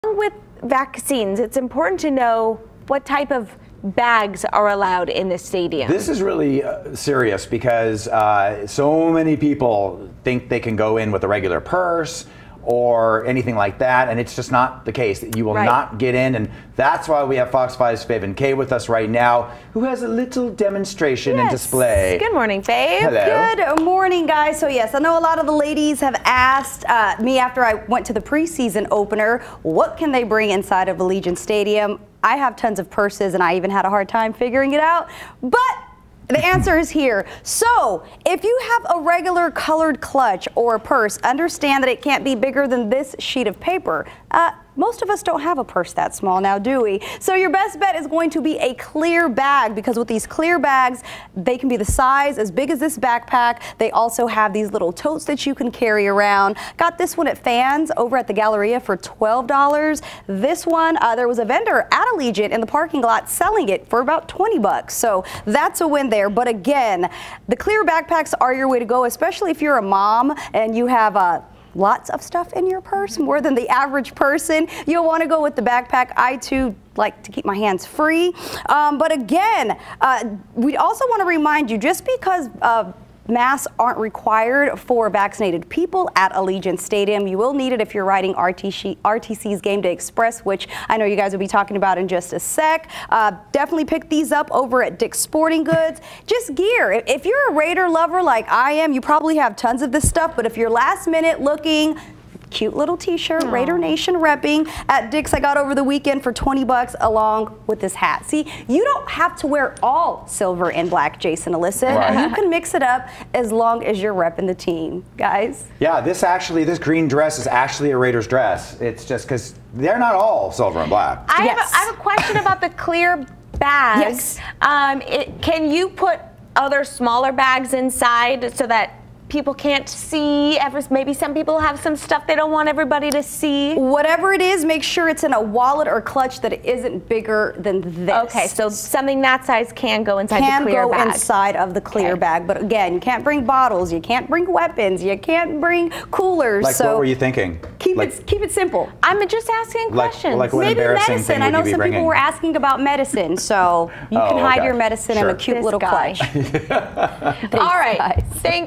Here's what to know about bringing bags to Allegiant Stadium
Going to a Raiders game at Allegiant Stadium? Here's what you need to know if you plan to bring a bag with you.